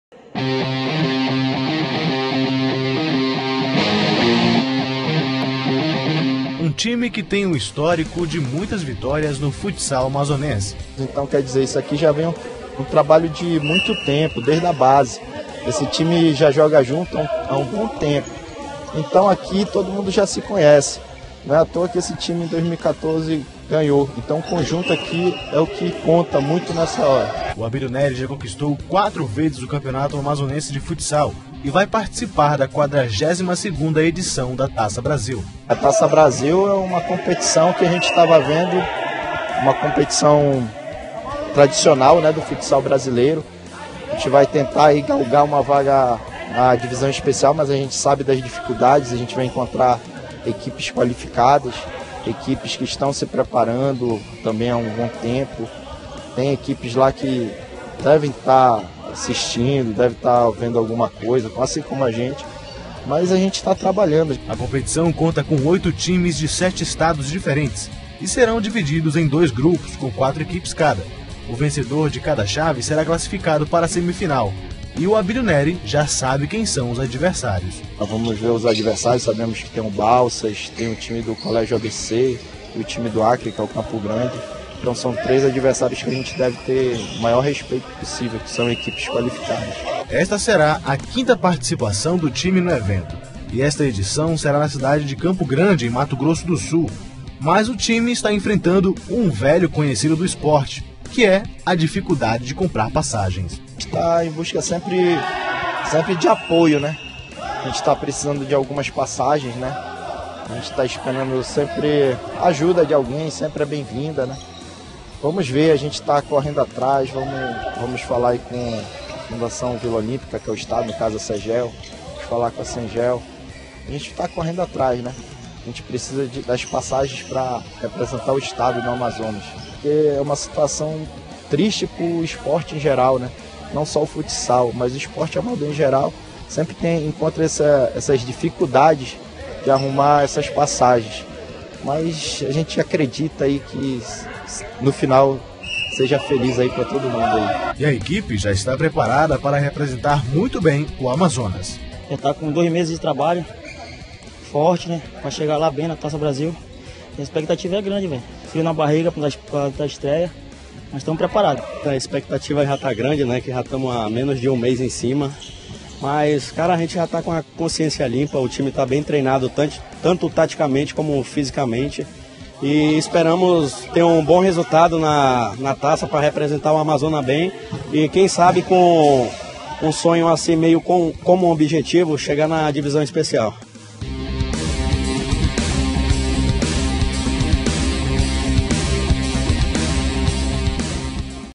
Esporte Amazonia | Representante do Amazonas na Taça Brasil em busca de apoio para viajar
Equipe amazonense que vai representar o estado na Taça Brasil de Clubes de Futsal, esta em preparação, mas ainda em busca de apoio para conseguir as passagens para toda equipe viajar.